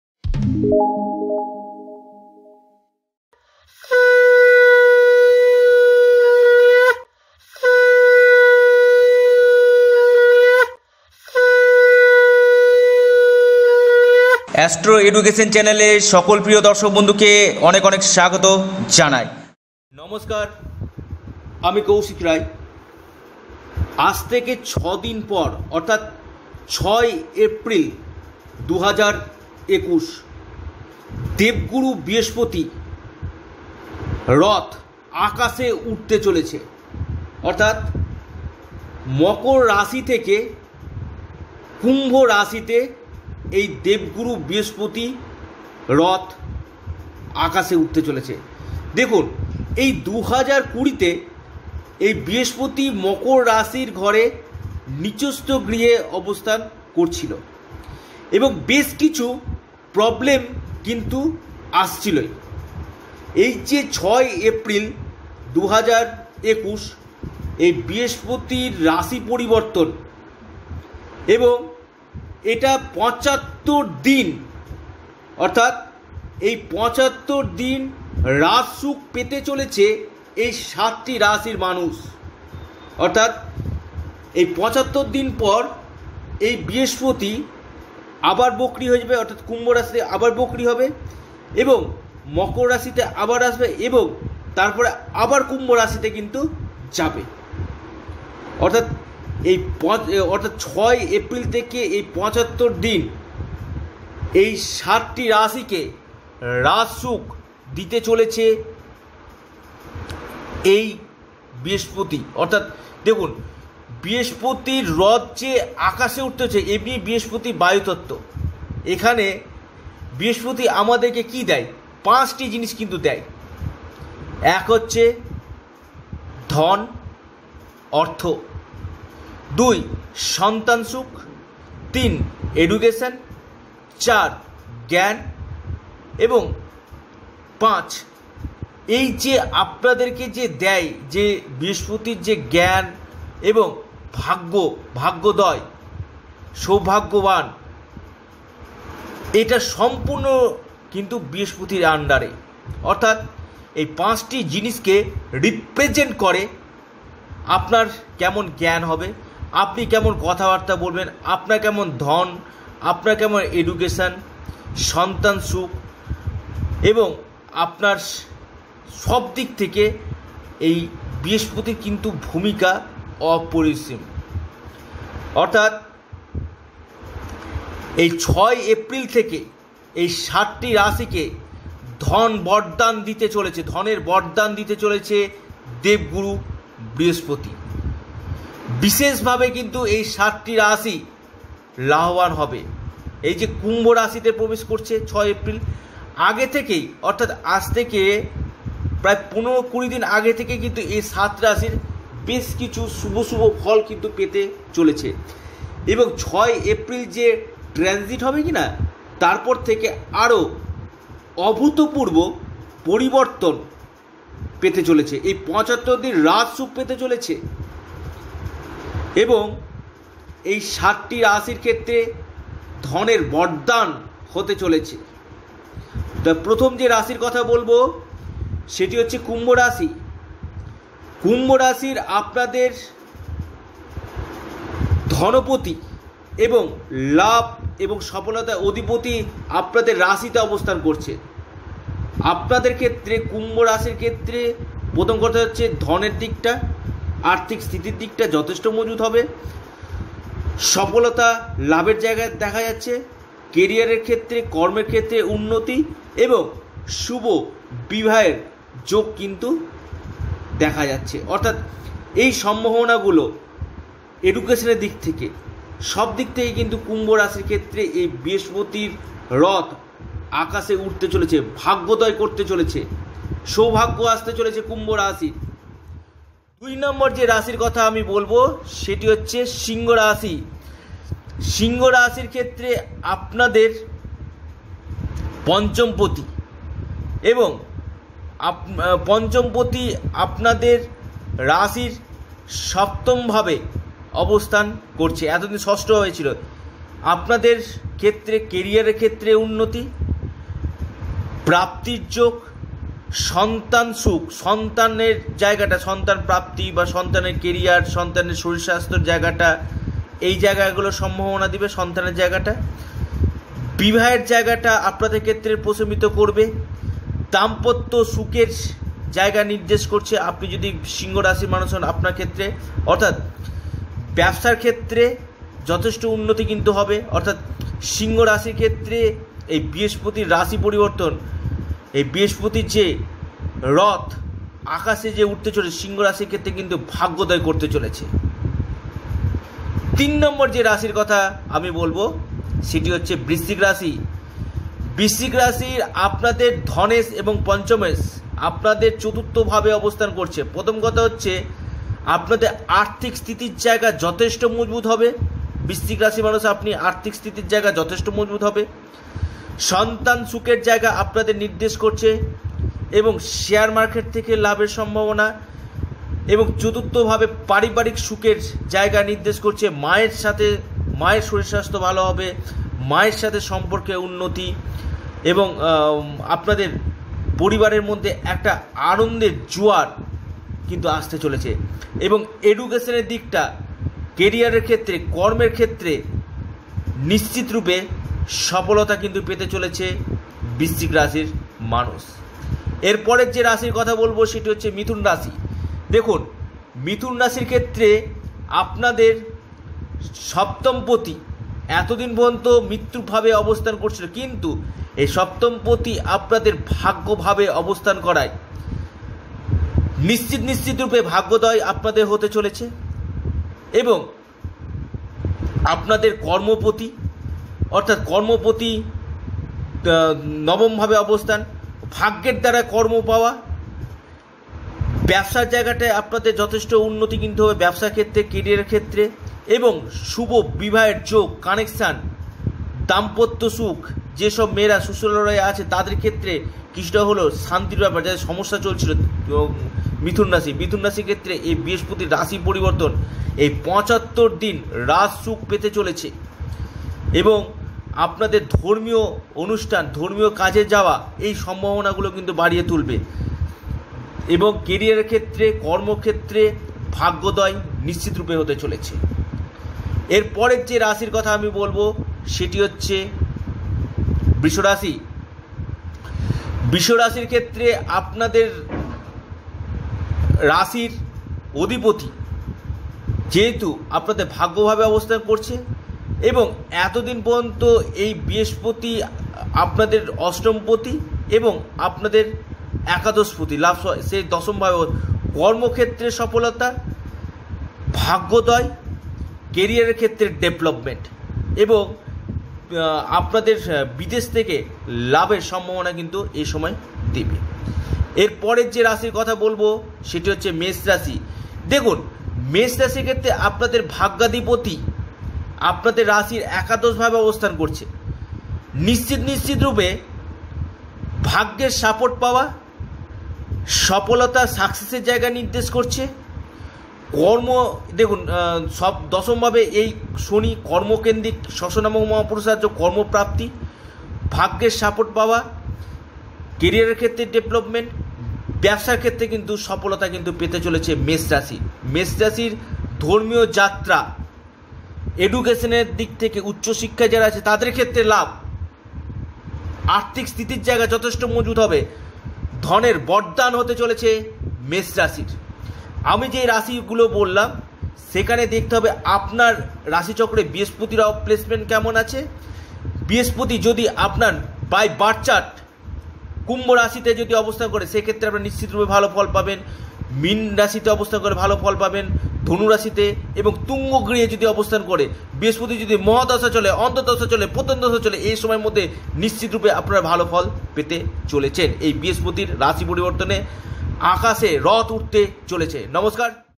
বৃহস্পতির রথ আকাশে উড়বে! এই 7টি রাশির ধন বরদান হতে চলেছে!
এই সাতটি রাশির বৃহস্পতি দেবের কৃপায় ধন বরদান হতে চলেছে এ বিষয় নিয়ে এই ভিডিওতে বিস্তারিত আলোচনা করা হয়েছে ধন্যবাদ!